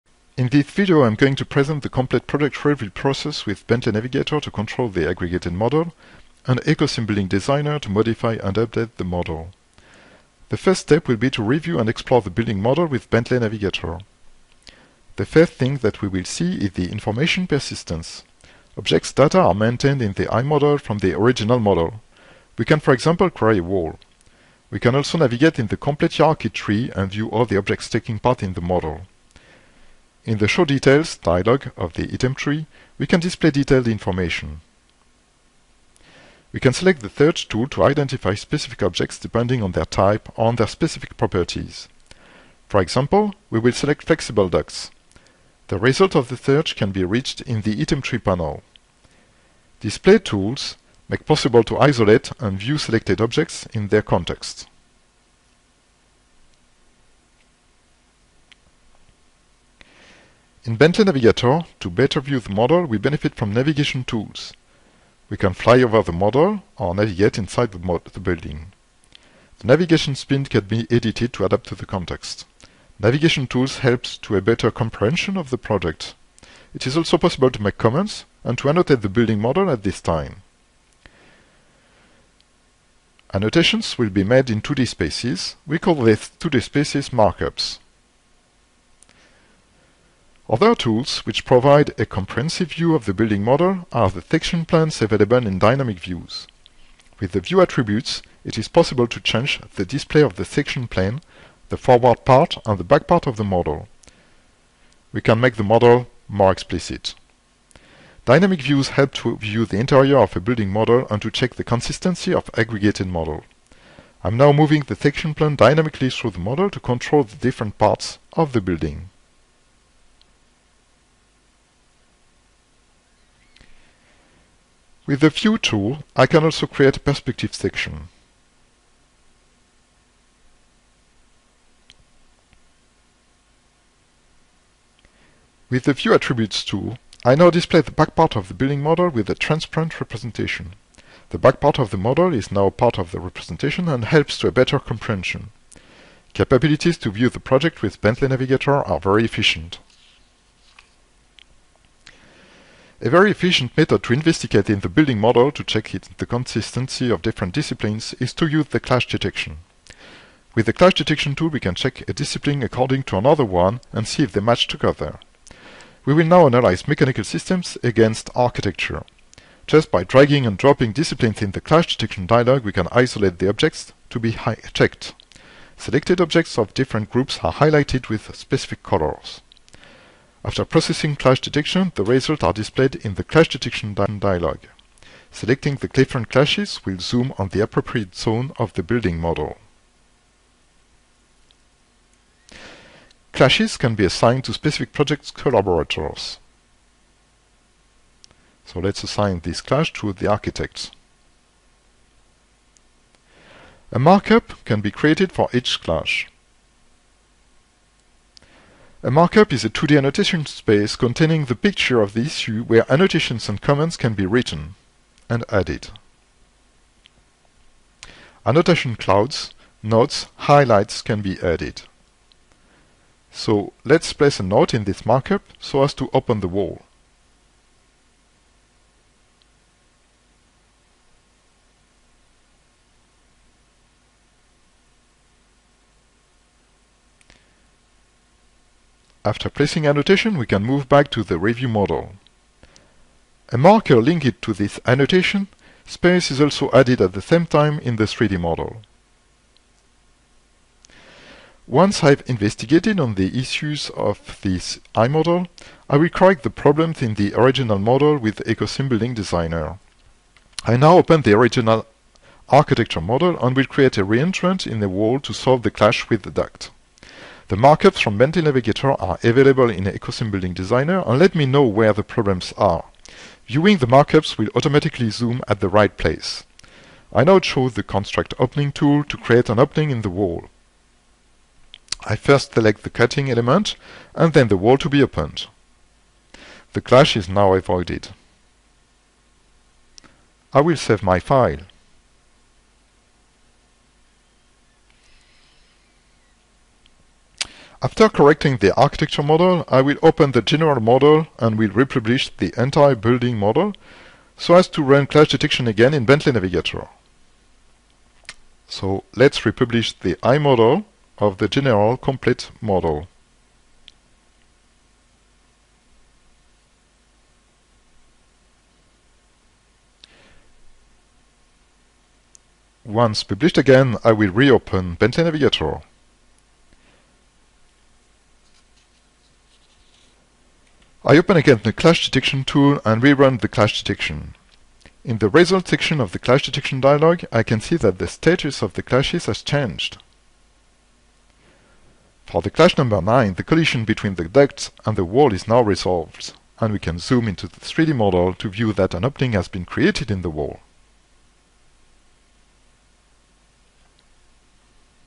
Project review with Bentley Navigator and AECOsimBuildingDesigner
Reviewing a building project with Bentley Navigator to check the project and with AECOsimBuildingDesigner to correct and modify the original 3D model